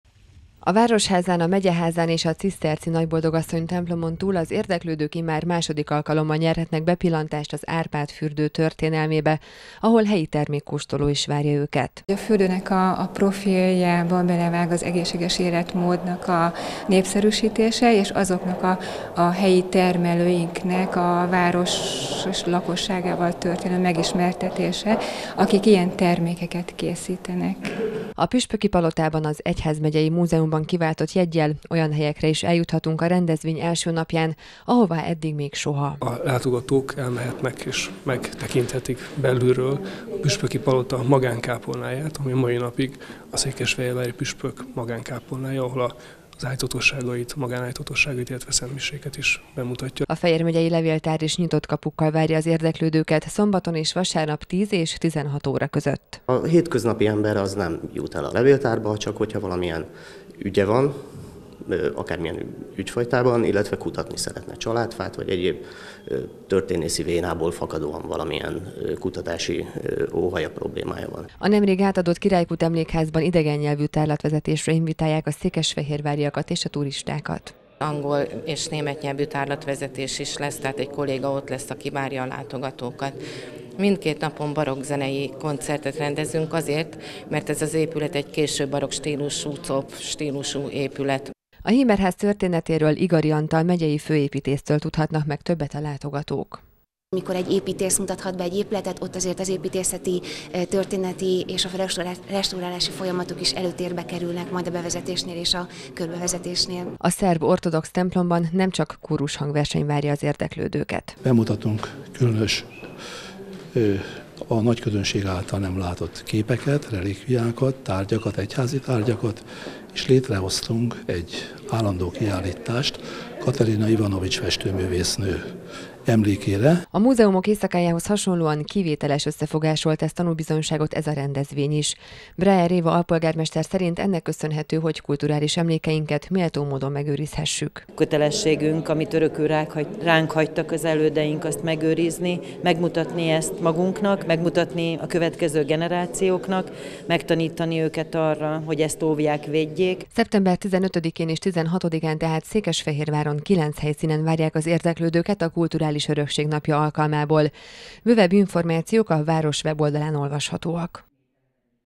Kulturális Örökség Napja
Országszerte ezer épület nyitja meg kapuit a Kulturális Örökség Napja alkalmából a hétvégén. Székesfehérváron idén is számos patinás épülettel és történetükkel ismerkedhetnek meg a látogatók, szakavatott vezetéssel.